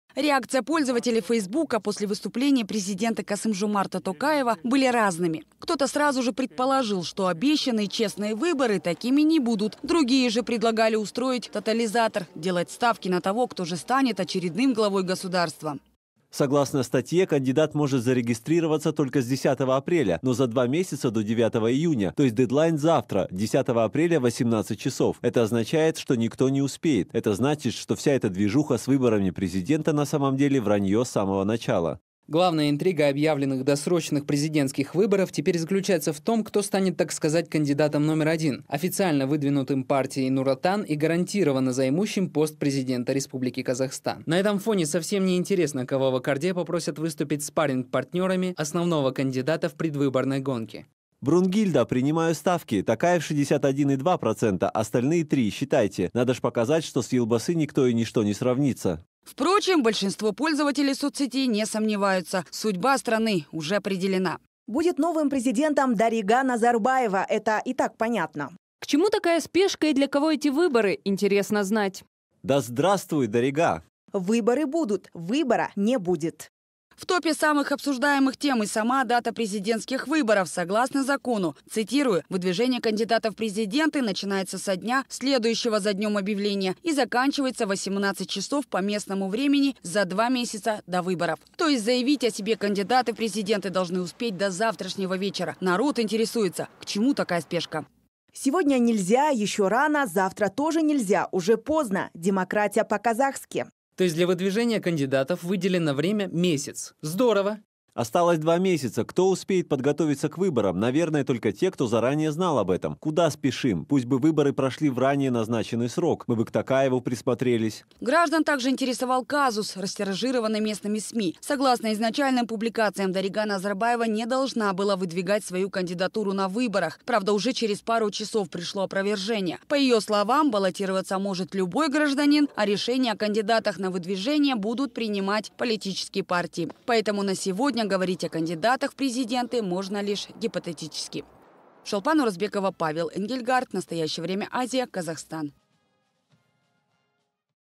«Выборы будут, выбора не будет»
Большинство избирателей задают вопрос: «Когда получит власть дочь Назарбаева Дарига?».